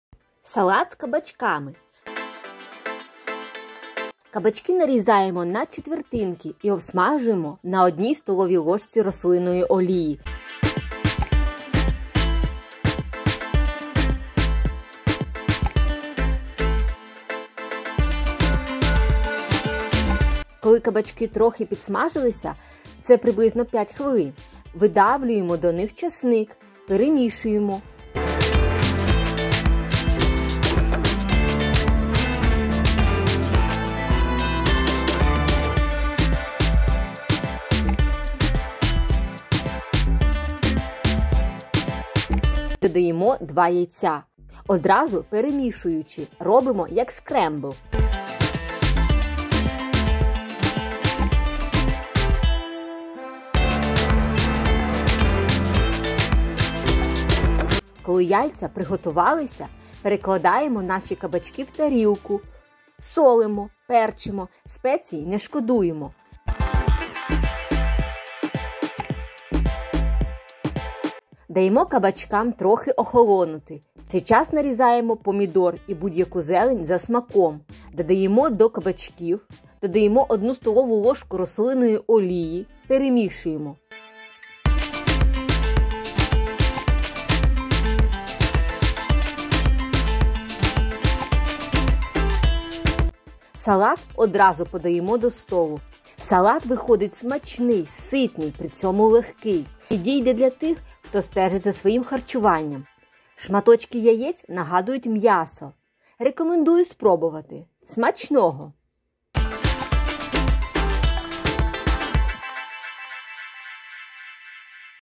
Салат з кабачками. Zucchini Salad
Салат з кабачками

Інгредієнти

Кабачки 2 шт (600 г)
Рослинна олія 2 ст. л.
Перець мелений за смаком
Часник 2-3 зубчики
Помідори 2-3 шт (200-250 г)
Свіжа зелень (листя салату, кріп, шпинат)

Кабачки нарізаємо на четвертинки і обсмажуємо на 1 столовій ложці рослинної олії. Коли кабачки трохи підсмажилися, це приблизно 5 хвилин, видавлюємо до них часник, перемішуємо і додаємо 2 яйця, одразу перемішуючи - робимо як скрэмбл. Коли яйця приготувалися, перекладаємо наші кабачки в тарілку, солимо, перчимо - спецій не шкодуємо. Даємо кабачкам трохи охолонути, в цей час нарізаємо помідор і будь-яку зелень за смаком, додаємо до кабачків, додаємо 1 столову ложку рослинної олії. Перемішуємо. Салат одразу подаємо до столу. Салат виходить смачний, ситний, при цьому легкий. Підійде для тих, хто стежить за своїм харчуванням. Шматочки яєць нагадують м'ясо. Рекомендую спробувати. Смачного!

Салат с  кабачками

Ингредиенты

Кабачки 2 шт (600 гр)
Перец молотый по вкусу
Чеснок 2-3 зубчика
Помидоры 2-3 шт (200-250 гр)
Зелень свежая ( листья салата, укроп, шпинат

Кабачки нарезаем на четвертинки и обжариваем на  1 столовой ложке растительного масла. Когда кабачки немного поджарились, это примерно 5 минут, выдавливаем к ним чеснок, перемешиваем и вбиваем на кабачки 2 яйца и сразу перемешиваем- делаем , как скрэмбл . Когда яйца приготовились, пересыпаем наши кабачки в тарелку, солим перчим- специй не жалеем. Даем кабачкам немного остыть, в это время нарезаем помидор и любую зелень по вкусу , добавляем к кабачкам , добавляем 1 столовую ложку растительного масла. Перемешиваем. Салат сразу подаем к столу. Салат получается вкусный, сытный, при этом легкий. Подойдет для тех кто следит за своим питанием. Кусочки яиц напоминают мясо. Рекомендую попробовать. Приятного аппетита.

Zucchini Salad

Ingredients

Zucchini 2 pieces (600 g)
Vegetable oil 2 tbsp
Eggs 2
Salt 1 tsp
Ground pepper to taste
Garlic 2-3 cloves
Tomatoes 2-3 pieces (200-250 g)
Fresh greens (lettuce leaves, dill, spinach)
Cut the zucchini into quarters and fry in 1 tablespoon of vegetable oil. When the zucchini is slightly browned, about 5 minutes, squeeze in the garlic, mix, and add 2 eggs, immediately stirring to make a scramble. When the eggs are cooked, transfer the zucchini to a plate, season with salt and pepper generously. Let the zucchini cool slightly, then chop the tomato and any greens to taste, add to the zucchini, and add 1 tablespoon of vegetable oil. Mix well. Serve the salad immediately. The salad is delicious, hearty, and light, suitable for those who watch their diet. The egg pieces resemble meat. Highly recommend trying it. Enjoy your meal!